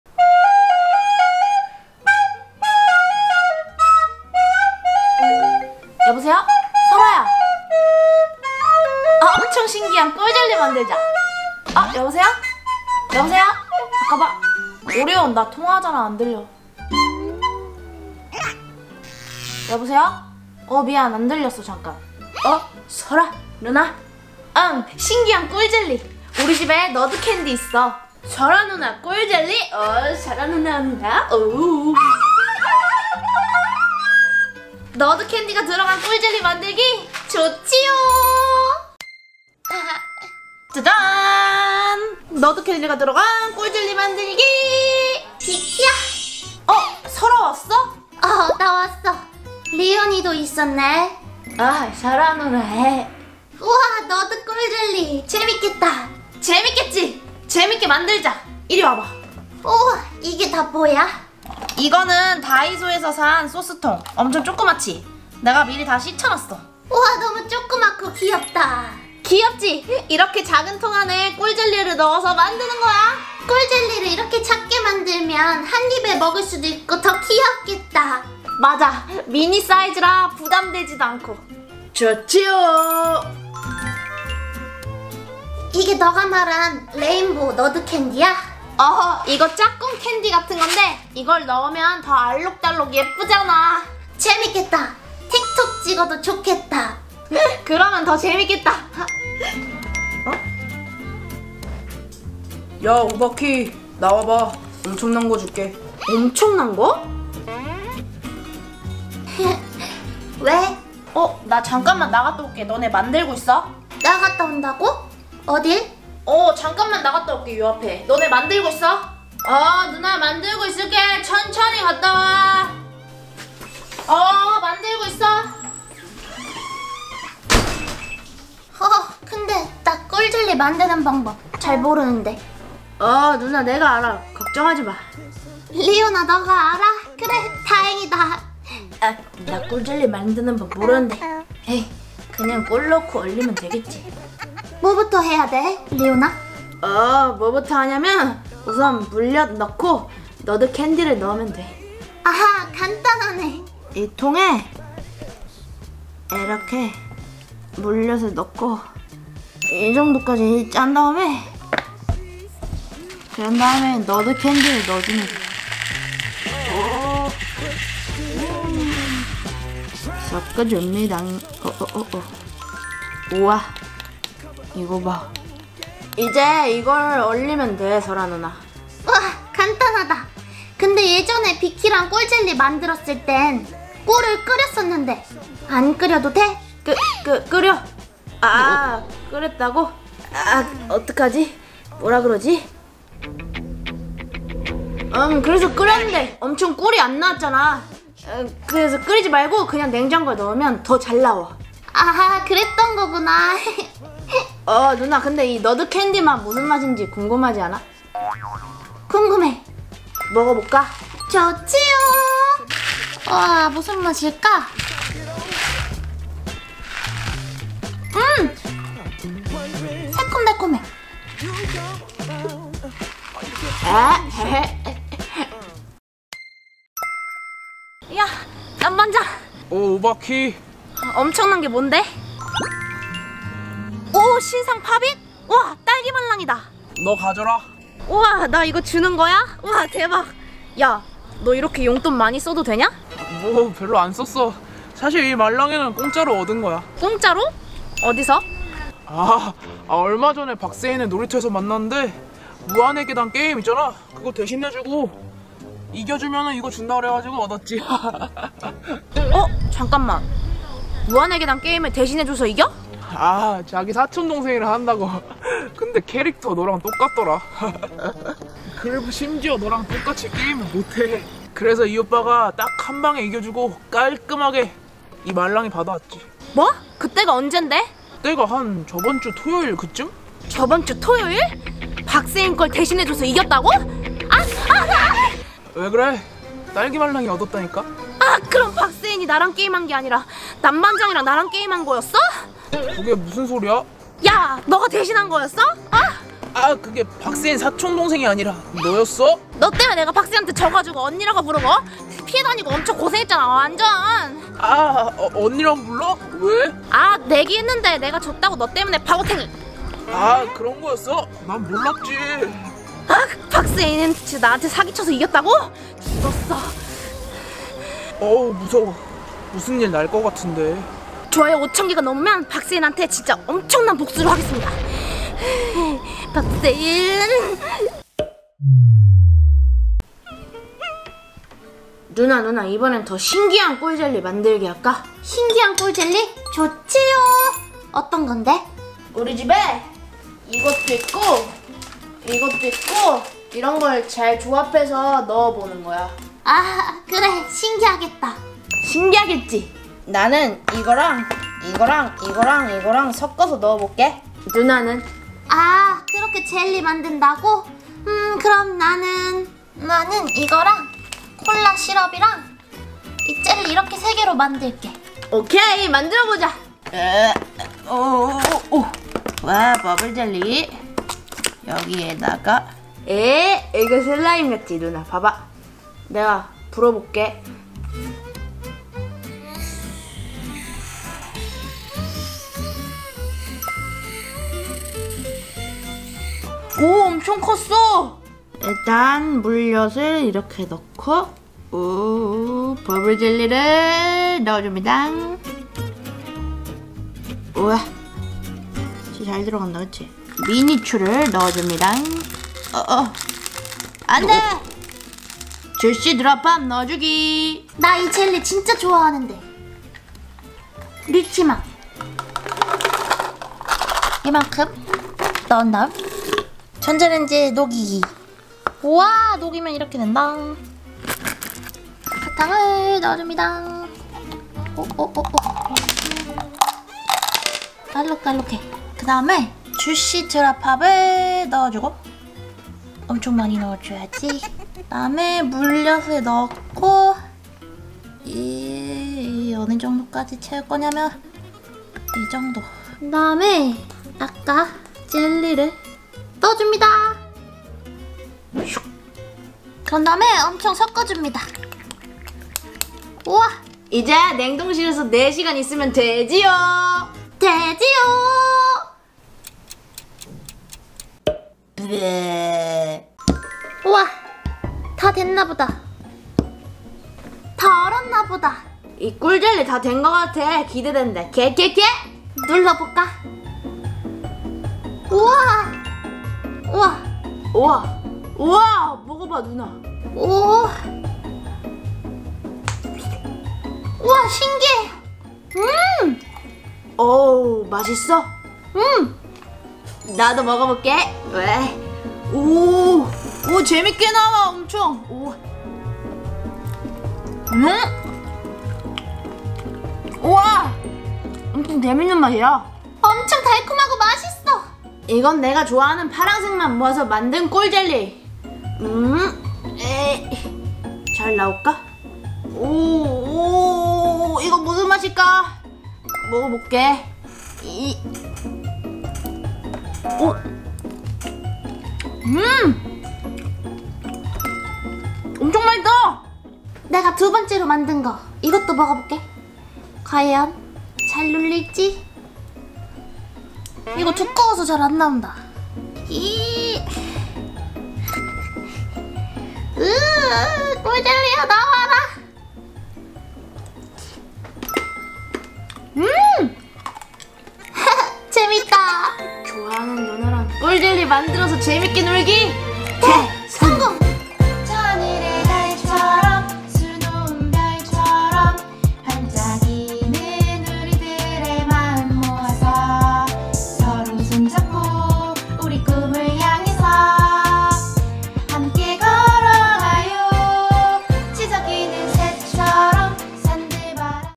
짝사랑이랑 신기한 꿀젤리 만들기 [오마이비키]
박세인의 사기가 발각 되었어요!
과연 오비키는 어떻게 복수를 할까요?